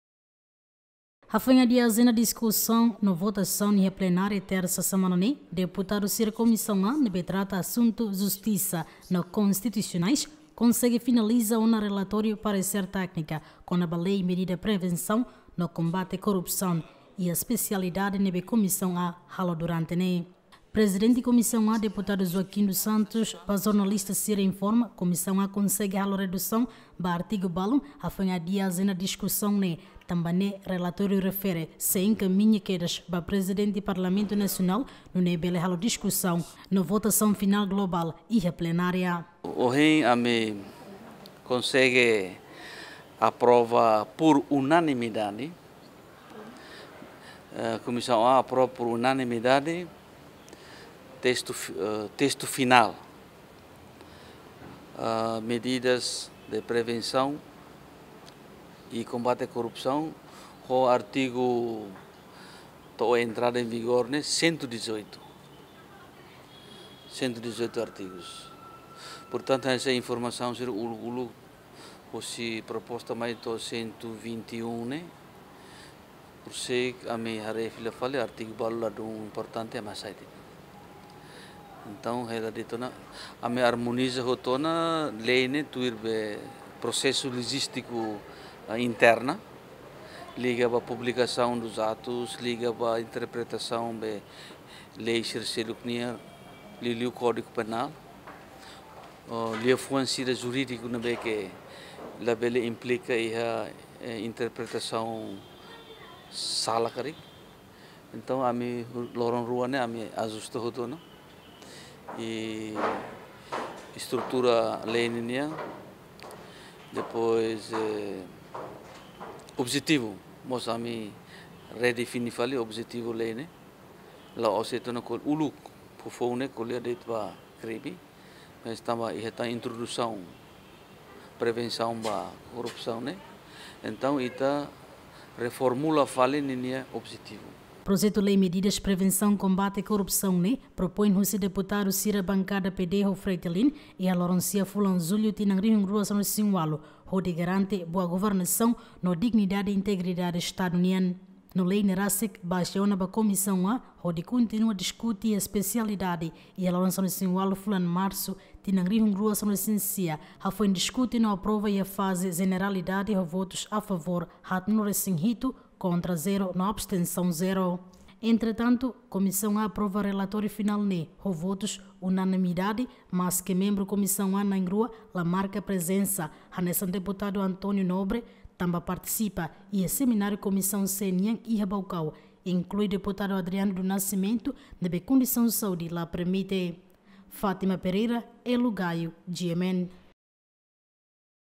Komisaun A PN Finaliza ona Relatóriu Pareser Téknika
Komisaun A PN Finaliza ona Relatóriu Pareser Téknika kona bá Lei Medida Prevensaun no kombate korupsaun